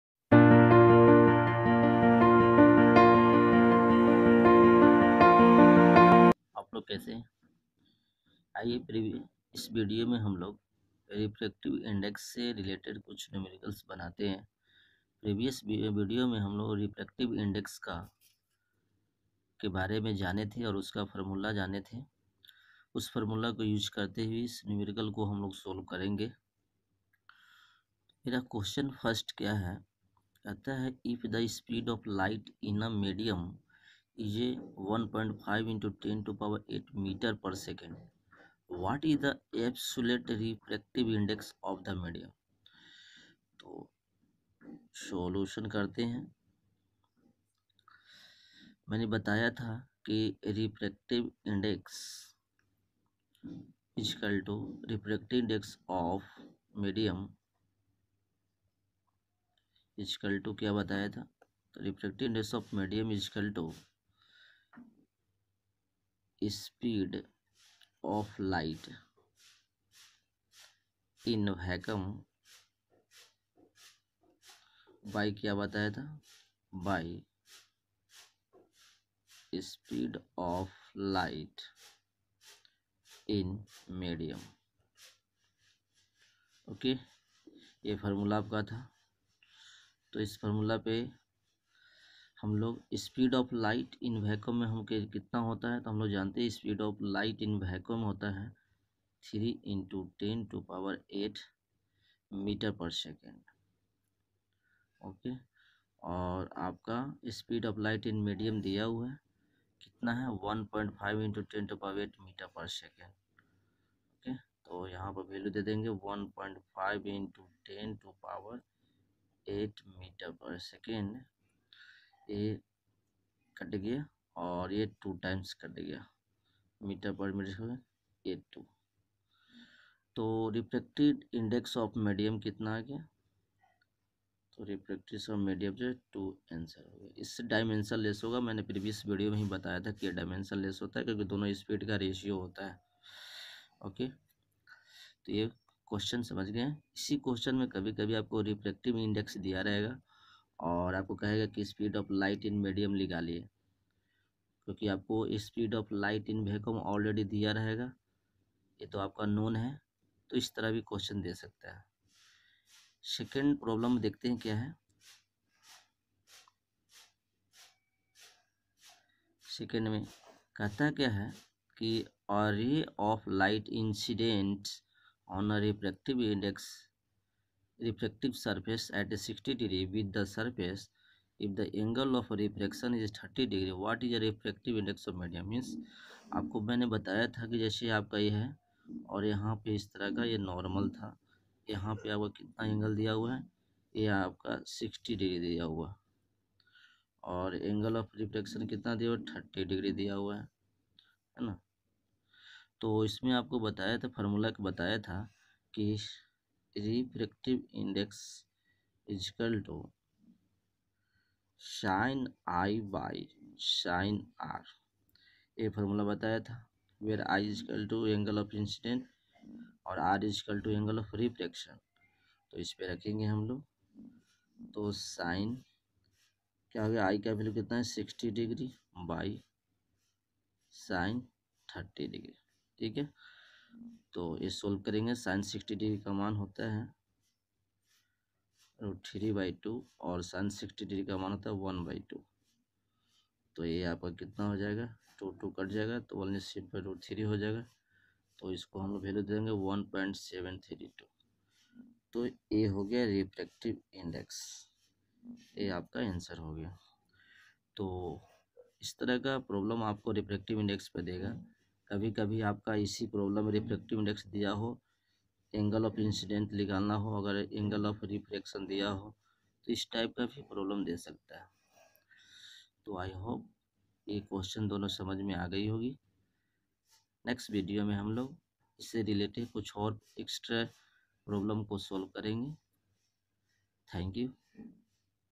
Absolute Refractive index class 10||Refraction of light| If Speed of Light in a medium is.What.
Hello students

In this video you learn how to find out Refractive index of When light travels one medium to another medium with some angles.

We use formulas

Absolute  R.I =  sini / sinr

Absolute R.I = speed of light in vaccum/speed of light in medium

physics teach and solve for class 10

Thanks